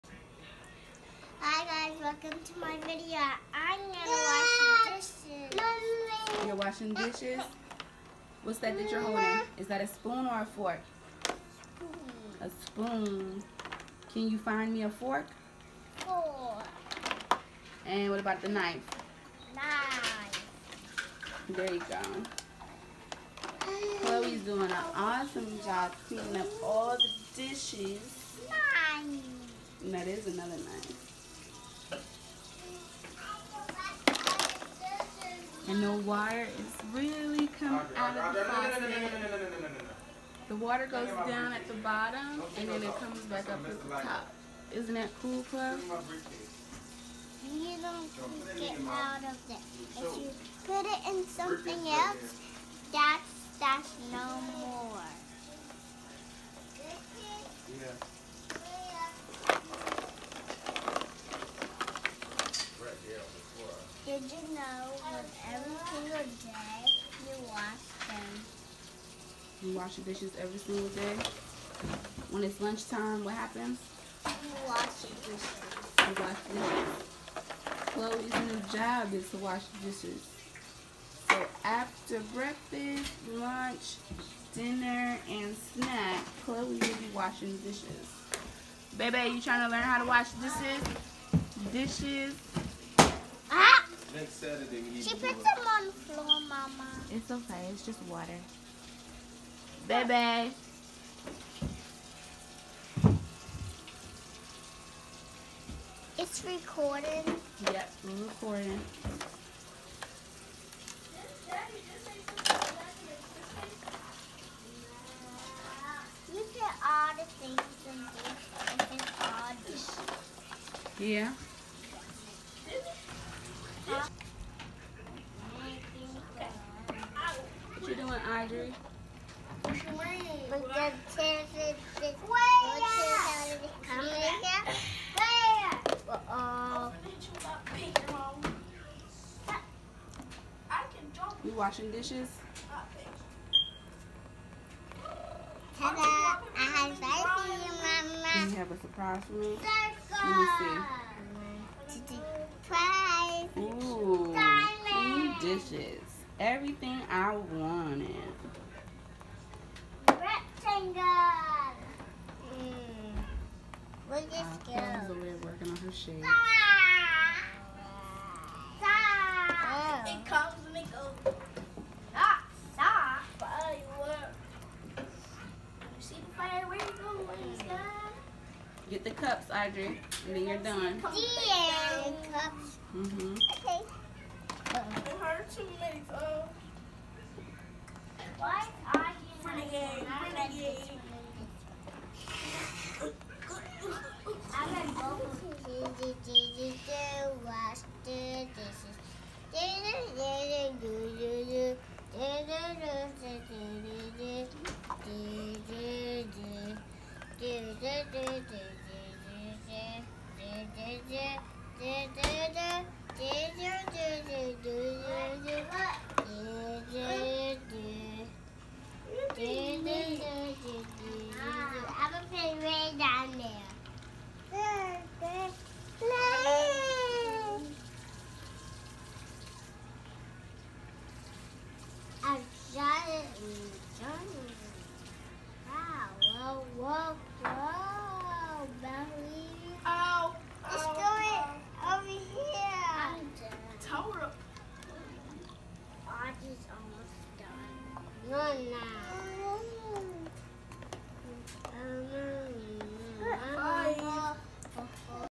Washing Dishes. (compiled version)
This is a compiled version of the girls, washing dishes. 😁